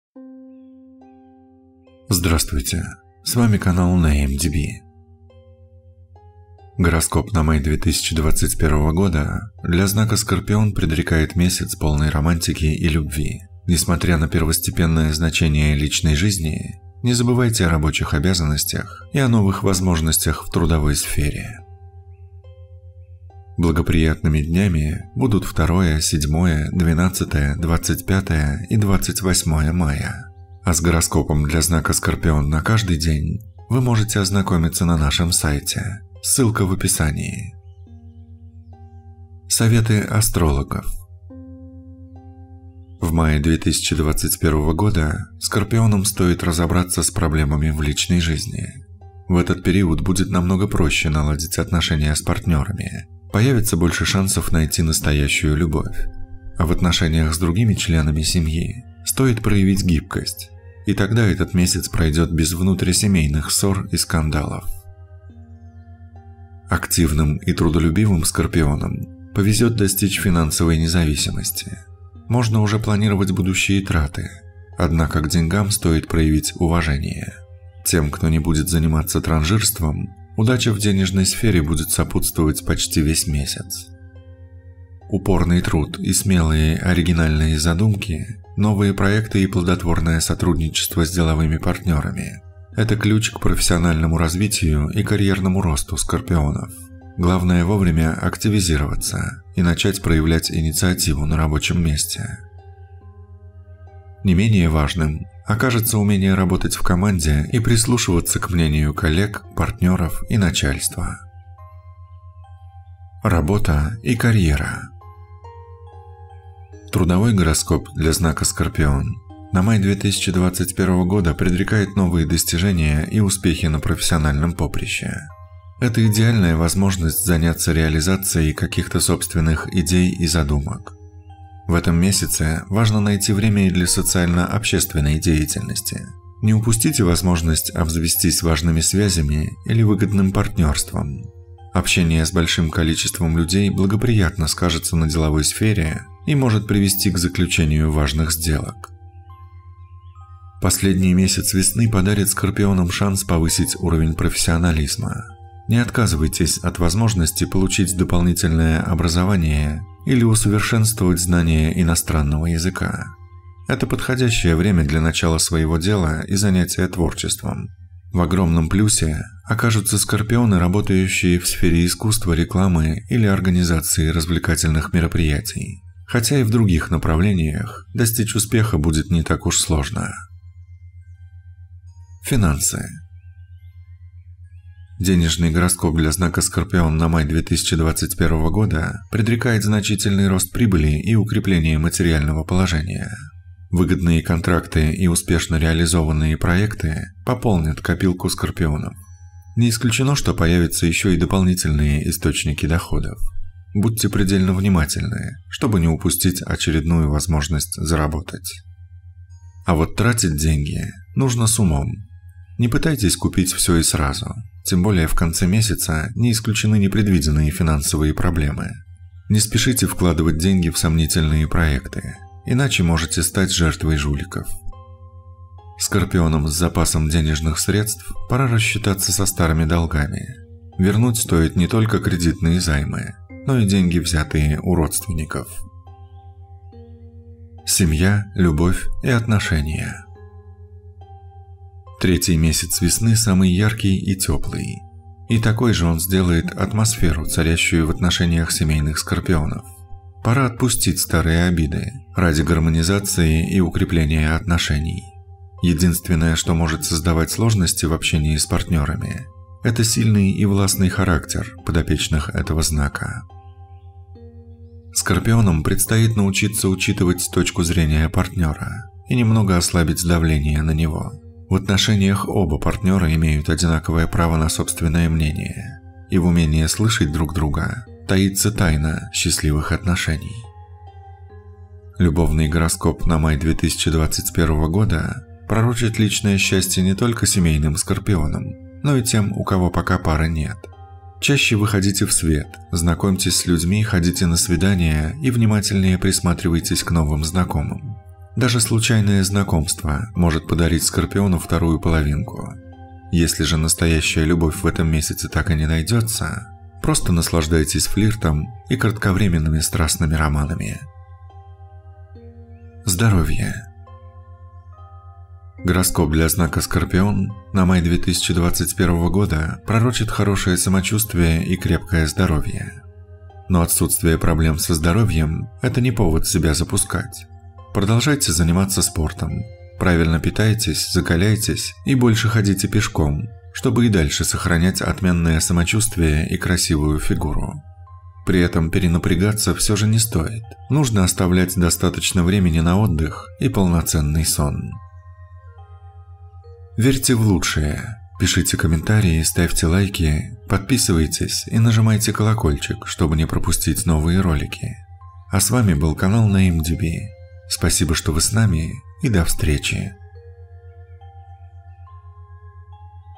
Знак Зодиака Скорпион - Гороскоп на Май 2021
Вместе с запахом весеннего тепла к Скорпионам придет вдохновение, хорошее настроение и желание изменить жизнь в лучшую сторону. Гороскоп для знака Скорпион на Май 2021 года предрекает месяц, полный романтики и любви. Несмотря на первостепенное значение личной жизни, не забывайте о рабочих обязанностях и о новых возможностях в трудовой сфере…

Благоприятные дни для Скорпионов в Мае 2021 года: 2, 7, 12, 25 и 28.

0:00 начало видео
00:38 Советы Скорпионам на Май
01:52 Работа и карьера
03:01 Деньги в жизни Скорпионов в Мае
03:56 Семья, любовь и отношения
05:11 Здоровье